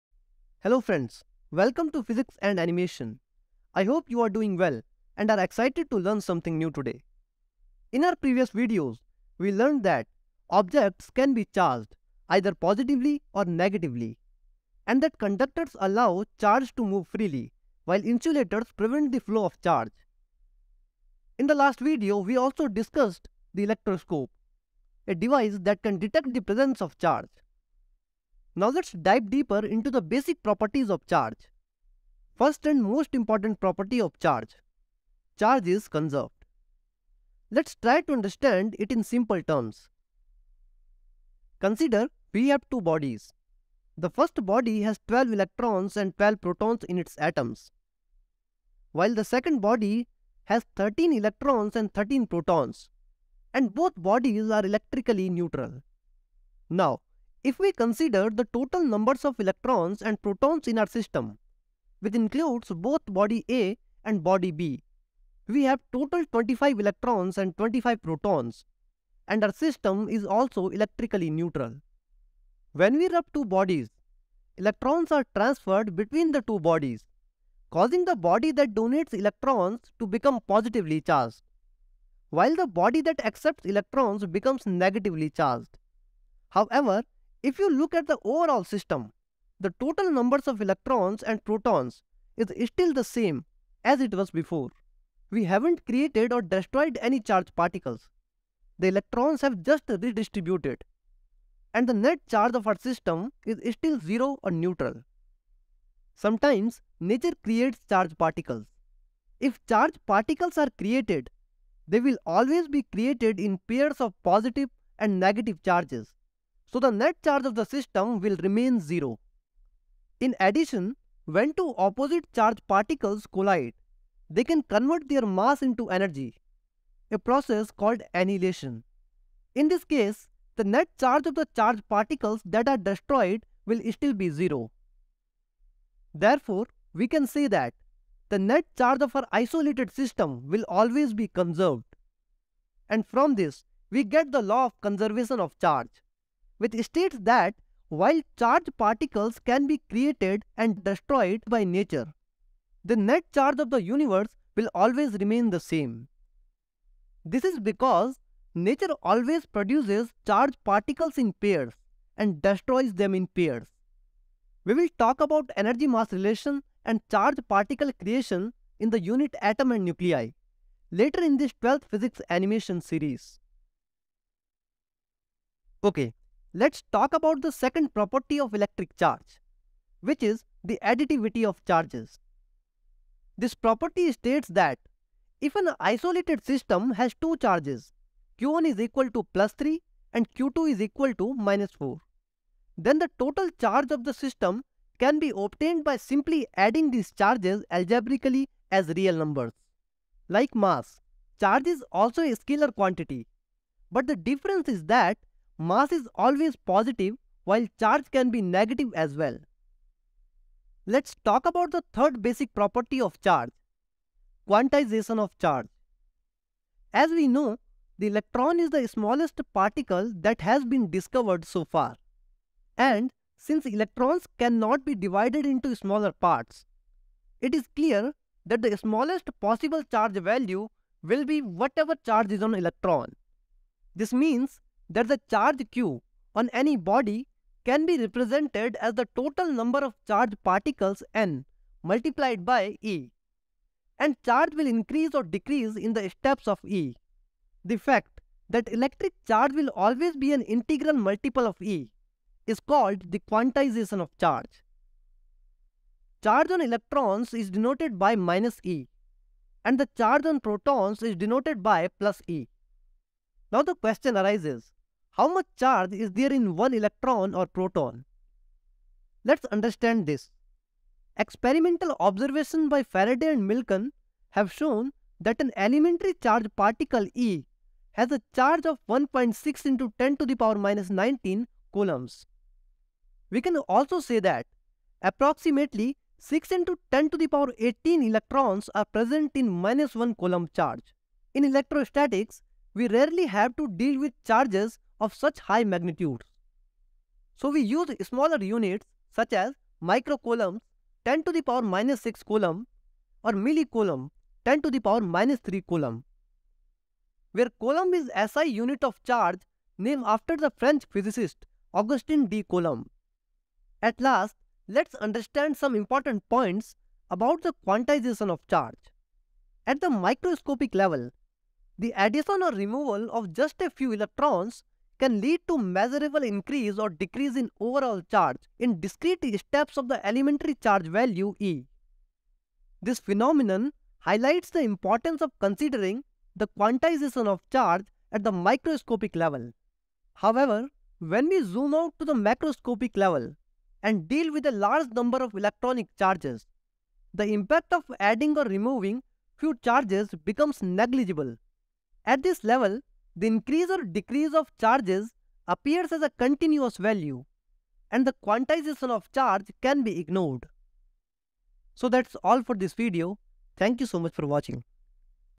Properties of Charges Explained with Animation for Class 12 Students
In this educational video designed for Class 12 students, we delve into an explanation of the properties of charges using engaging animations. Understanding the fundamental concepts of charges is crucial for mastering the subject, and with the help of visual aids, we aim to simplify the complex ideas for easier comprehension. Watch as we break down the properties of charges in a way that is both informative and visually stimulating, making learning fun and effective for students.

*12th Physics Animation Series-:*

Queries Solved-:
1. 0.0 - Intro
2. 0.27 - Charge is conserved
3. 2.56 - Additivity of charge
4. 3.31 - Quantization of Charge
5. 5.06 - important point ( microscopic and macroscopic approach in quantization of charge)